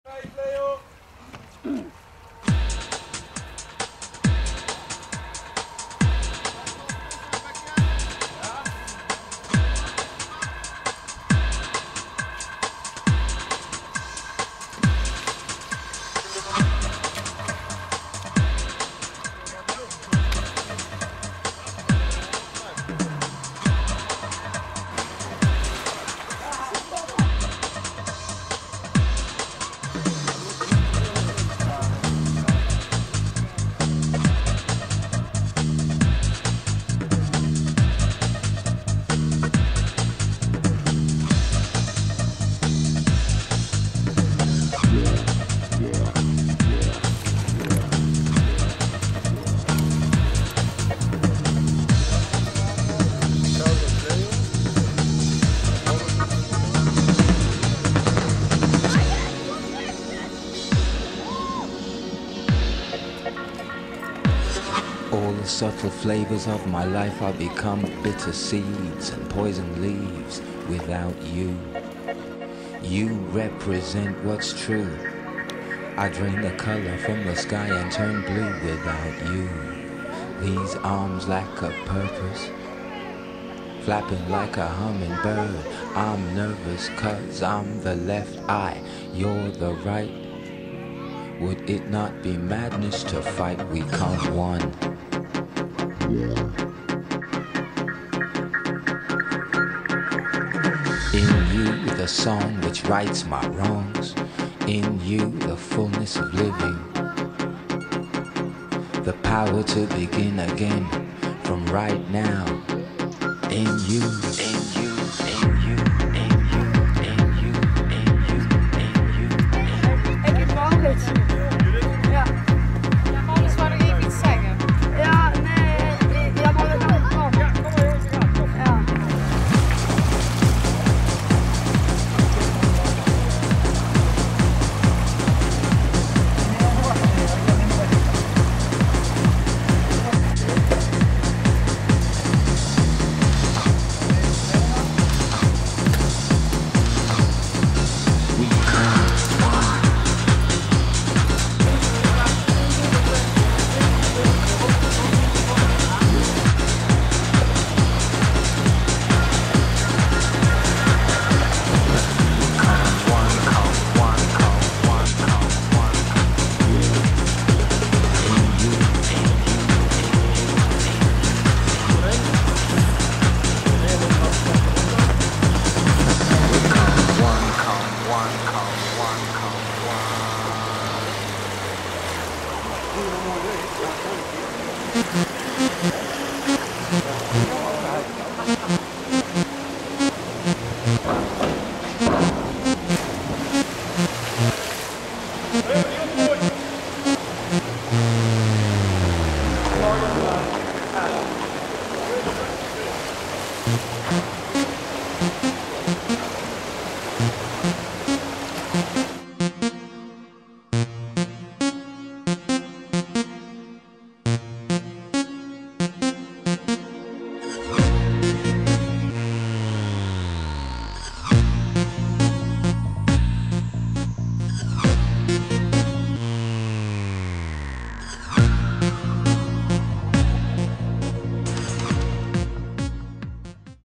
Duikoefening brandweer Helmond C ploeg
duikoefening op recreatie plas en in het zwembad